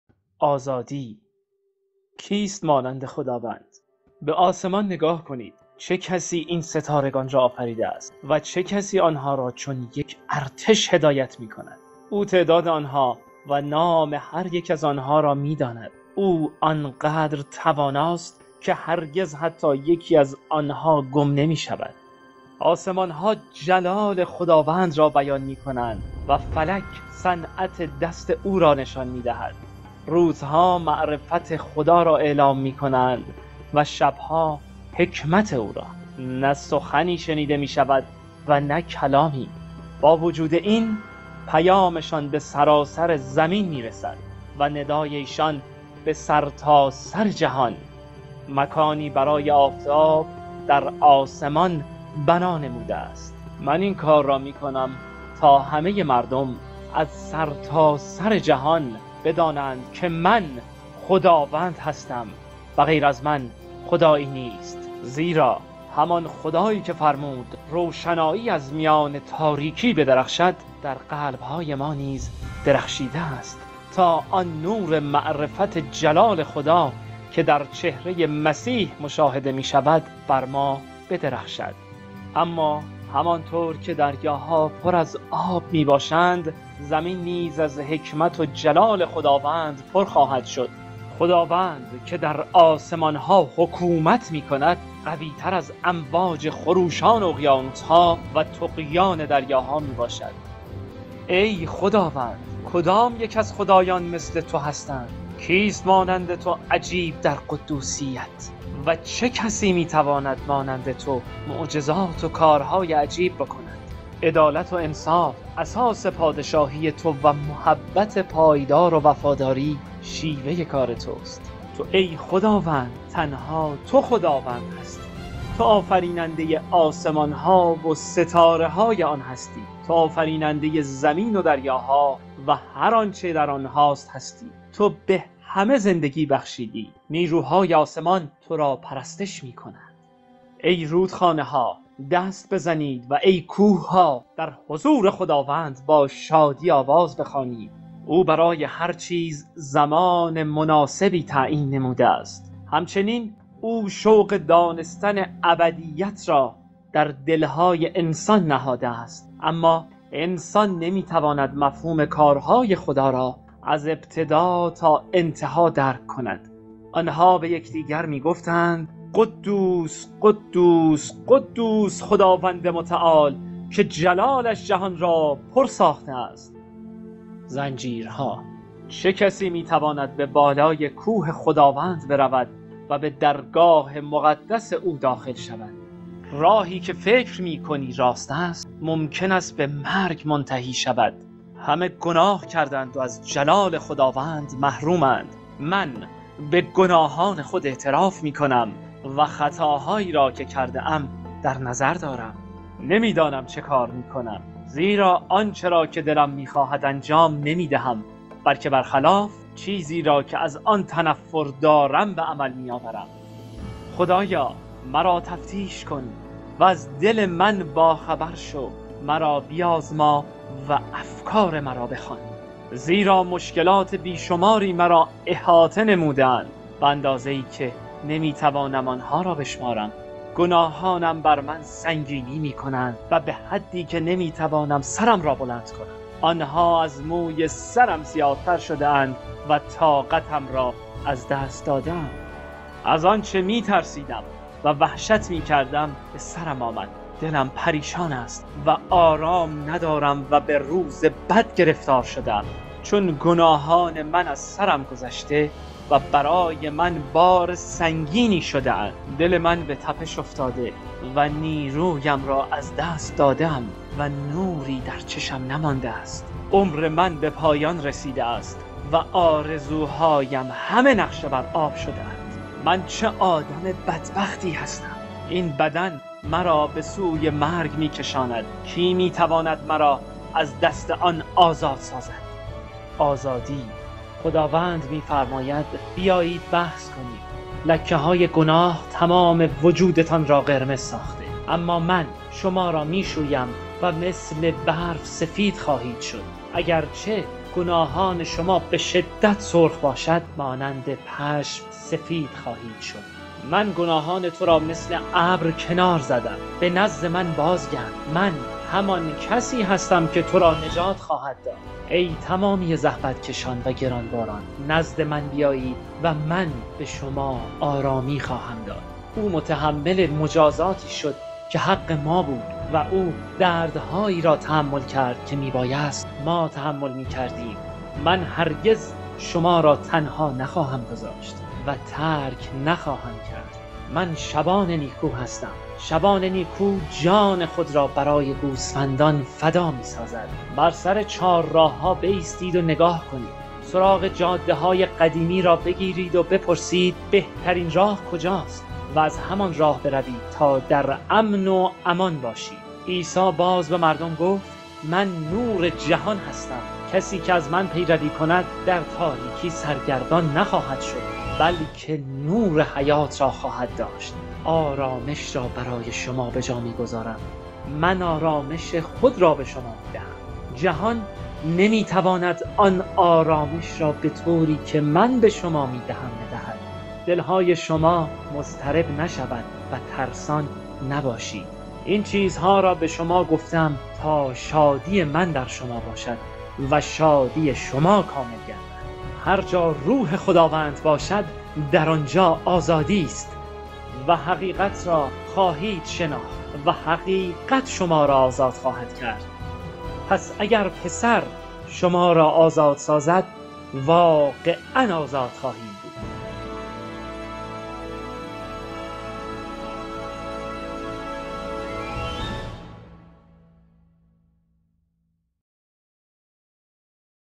Freedom_ On Every Word
On Every Word Ministries, and Iran Alive Ministries, entered into a partnership together to help the 6 Mil unique viewers of Shabakeh 7, Iran Alive Ministries Network in Iran to hear and proclaim the Word of God.
A special thanks to Tim McKenzie with On Every Word Ministries for having a heart for other nations with different languages to hear, see, proclaim, and understand the powerful Word of God through multimedia.